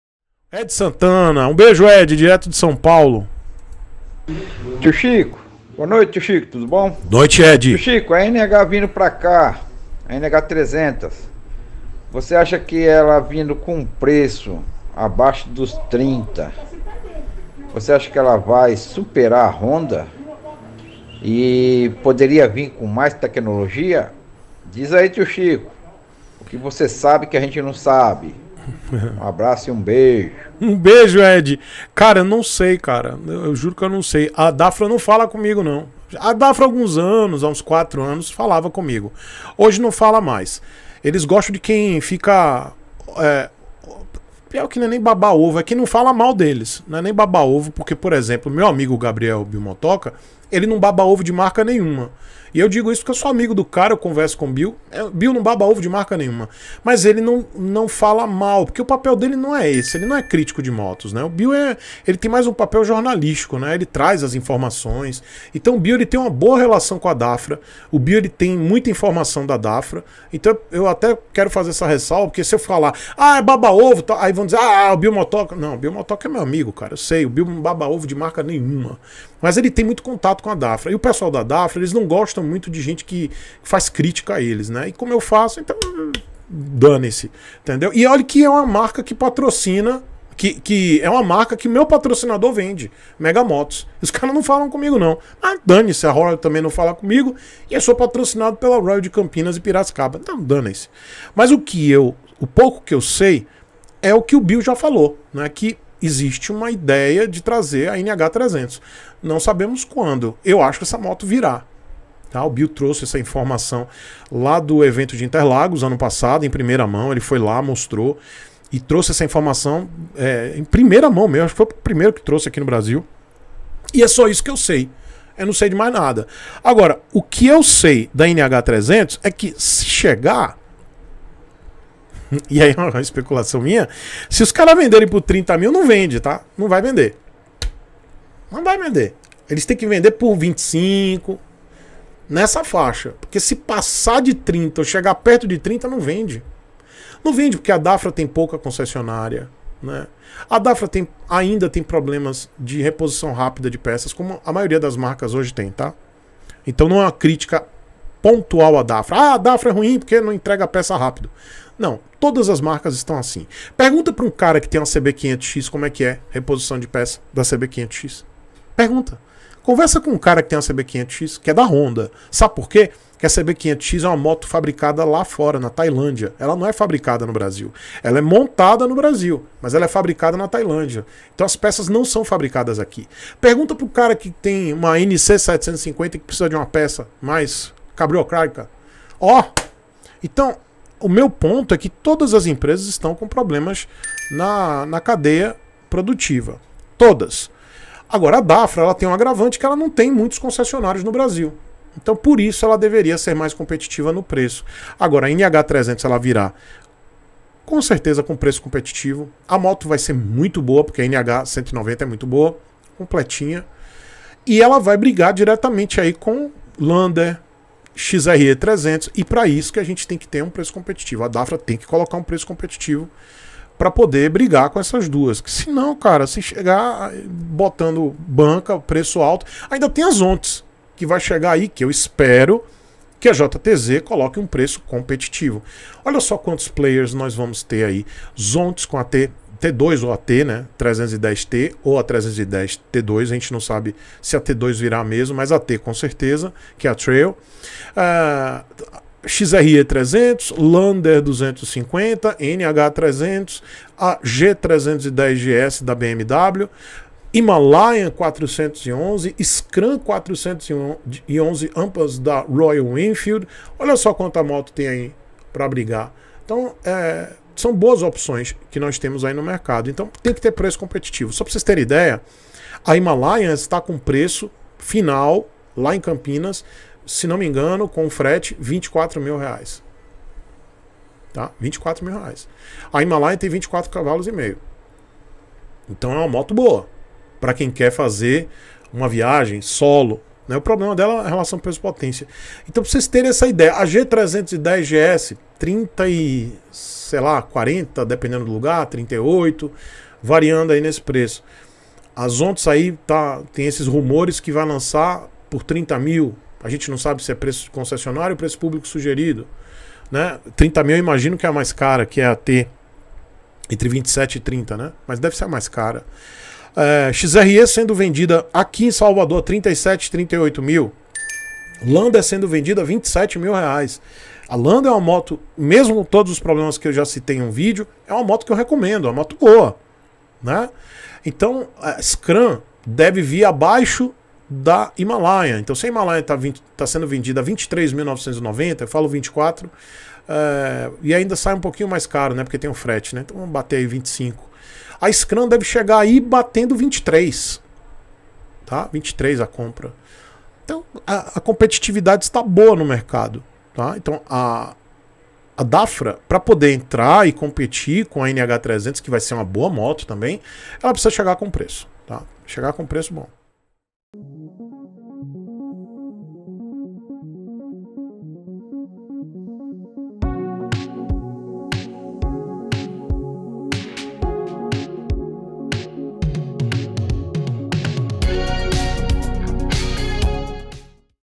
Dafra NH300 pode superar Yamaha Lander 250 ou Honda XRE300?
Black Boots
Calçados com estilo, conforto e segurança